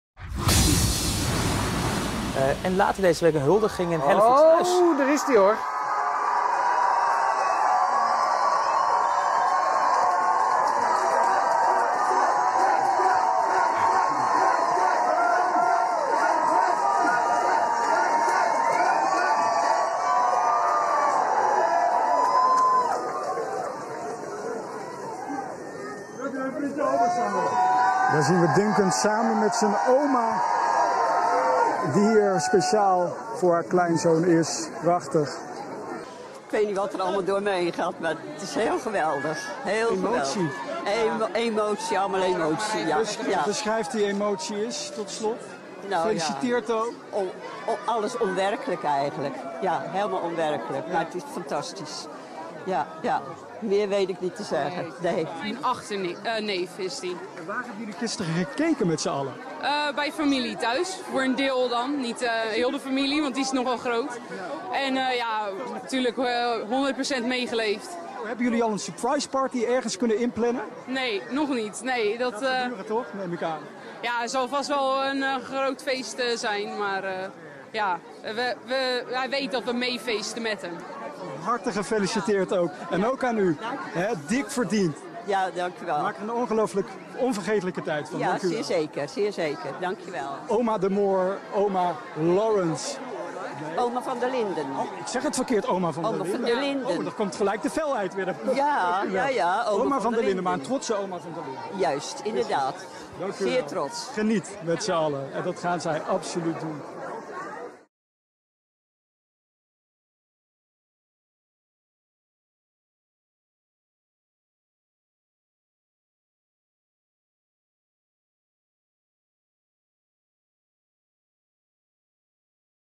Stralende Duncan arriveert op Schiphol
Duncan Laurence is na zijn overwinning van het Eurovisie Songfestival terug in Nederland.